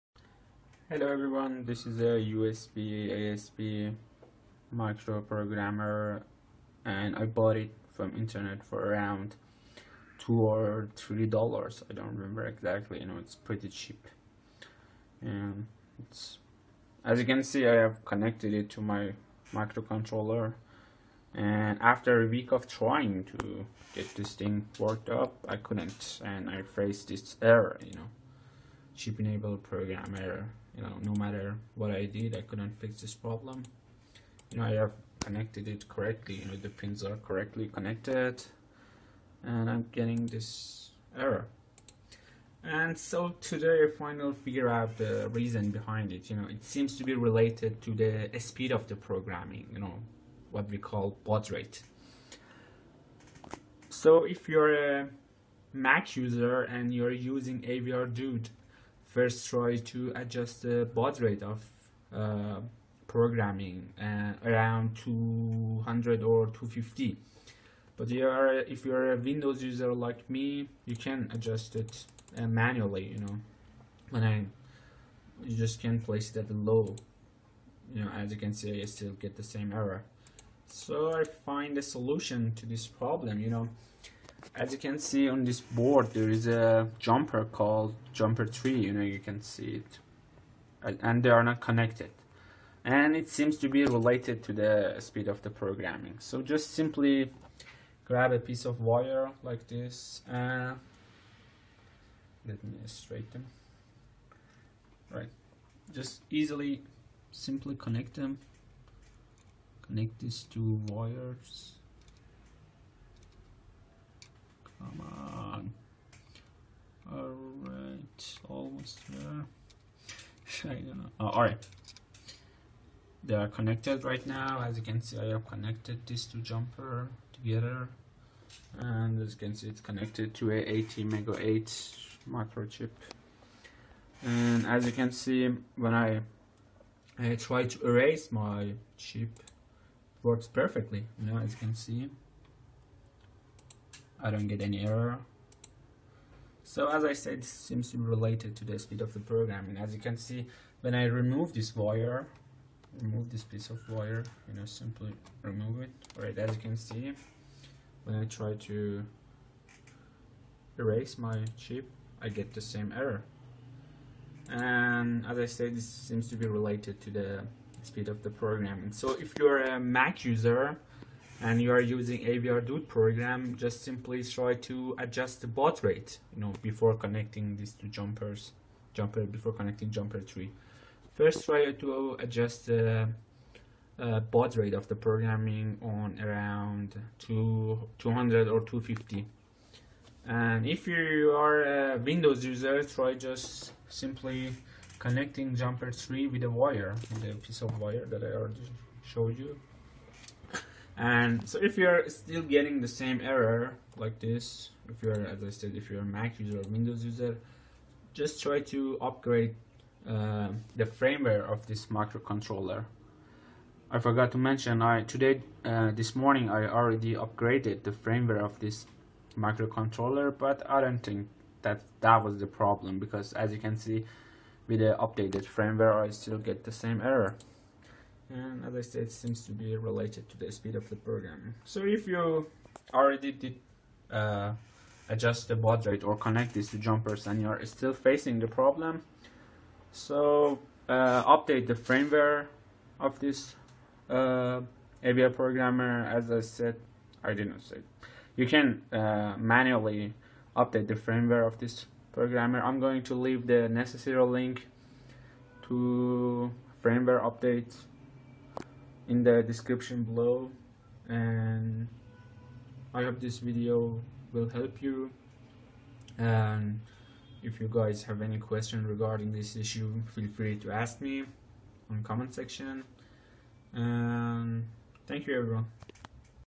how to fix chip enable program error on USBasp programmer
in this video,i am going to show you how to fix chip enable program error on  USBasp programmer for windows and mac users.//
if you already tried to adjust baud rate on avr  dude(mac),or connected jumper3 and still getting the same error,try updating frame ware,and then adjust the baud rate (mac),or connecting jp3(windows).